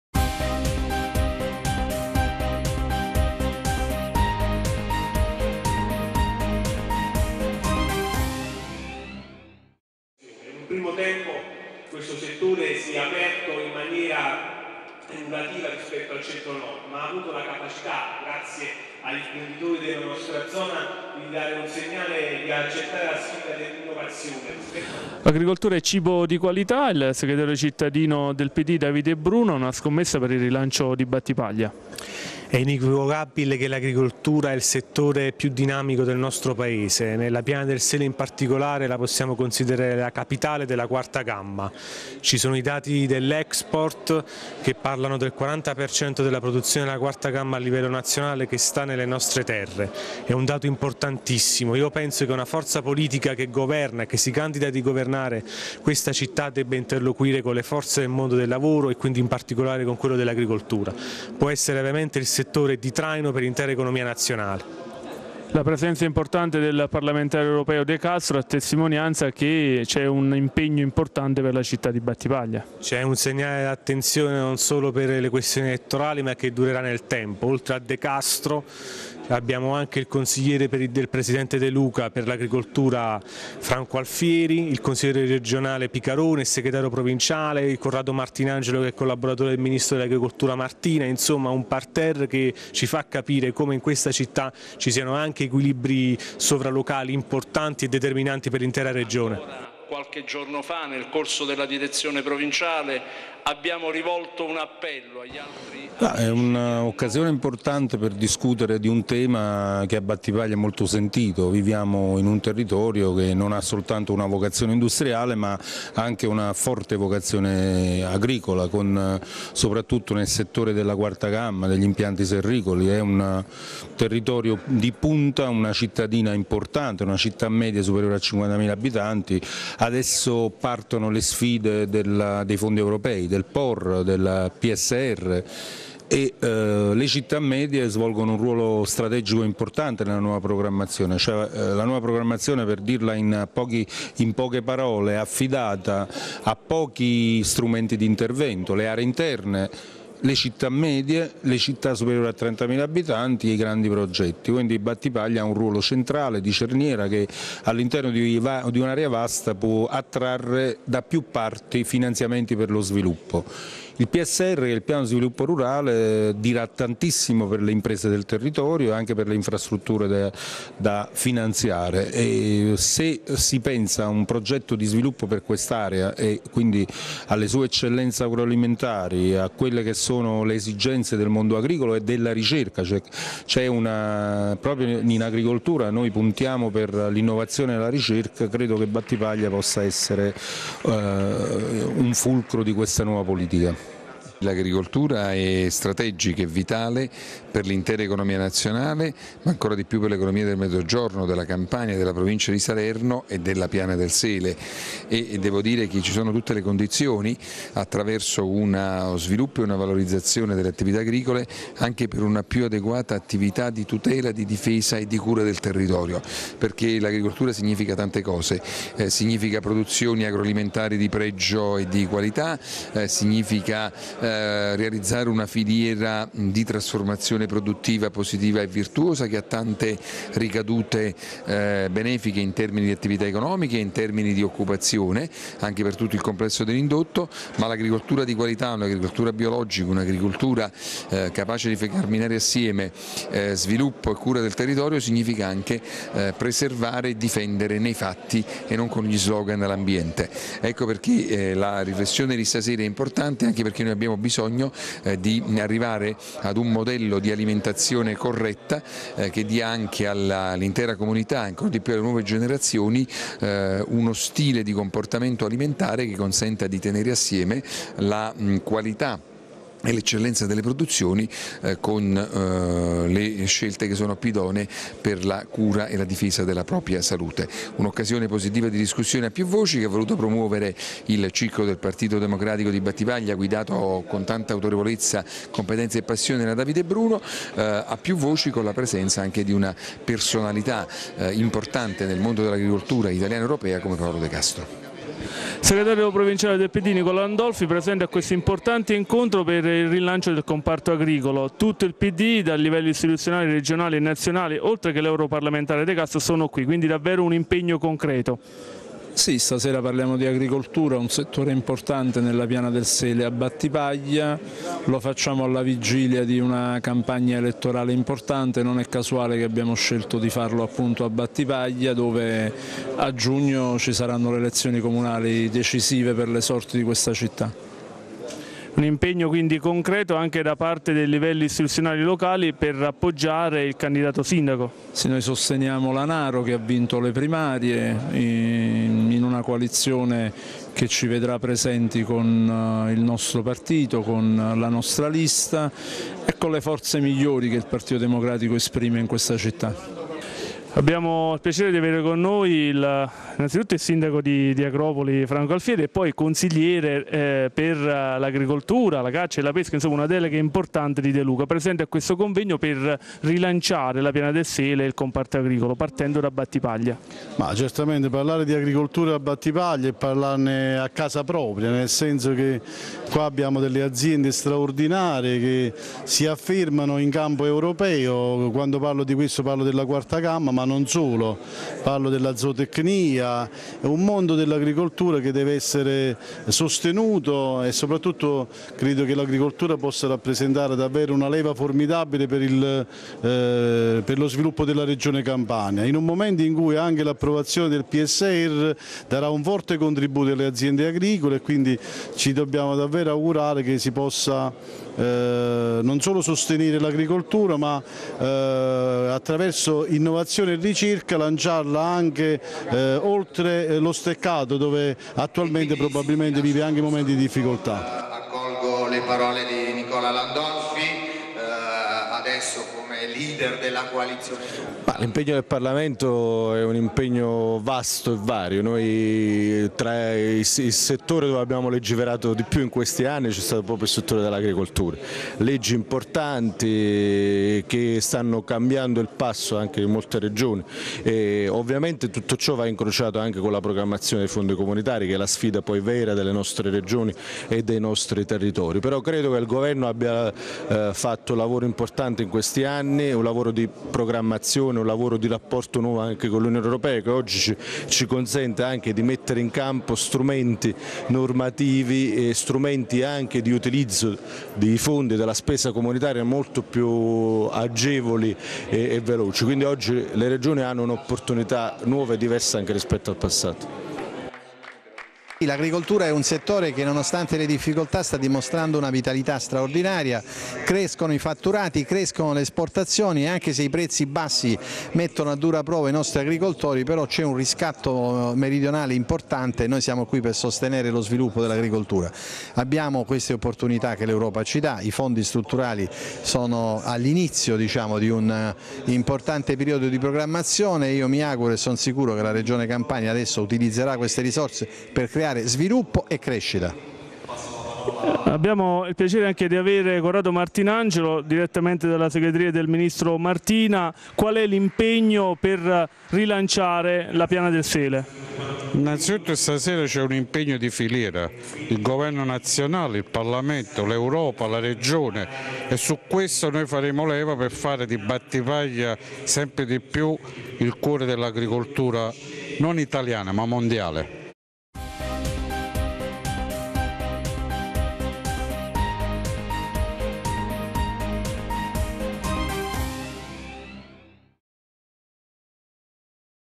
SPECIALE TG NEWS - BATTIPAGLIA: AGRICOLTURA E CIBO DI QUALITA'
Convegno organizzato e promosso dal gruppo dell’Alleanza Progressista  dei Socialisti e Democratici del Parlamento Europeo, presso il Salotto Comunale di Battipaglia. Filiera tra comune, regione, parlamento Italiano ed Europeo, per rilanciare i fondi strutturali europei in agricoltura ed i Por regionali.